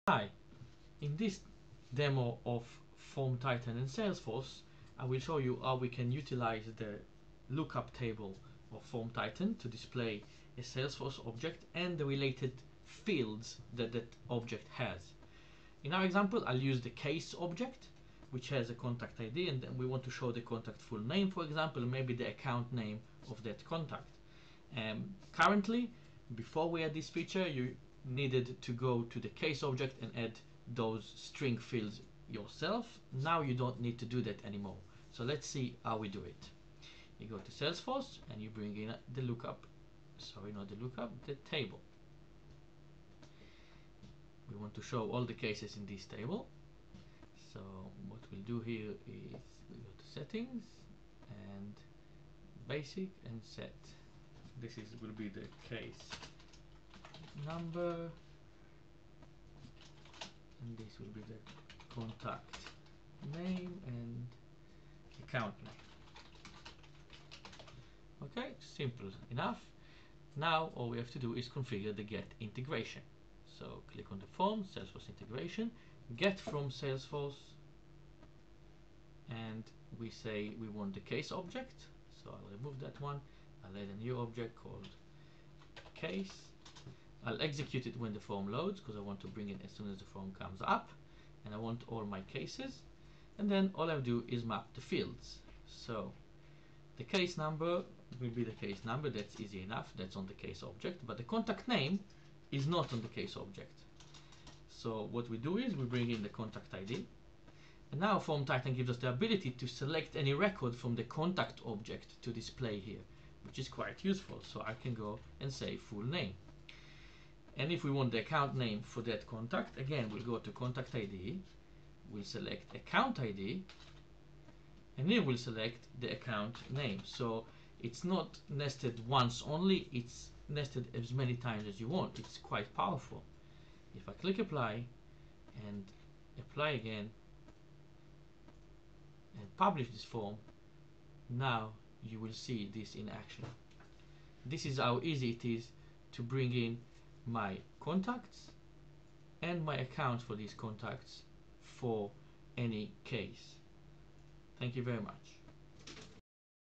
Use Salesforce lookup fields to get the related object on a digital form
In this video we will show you how you can use a digital form to get the case object from Salesforce and map it's fields to a table.
We will map the lookup field contact id to contact (fullname) and to account by using the contact id and account id.

This enables you to display any related object's value on the Salesforce lookup table.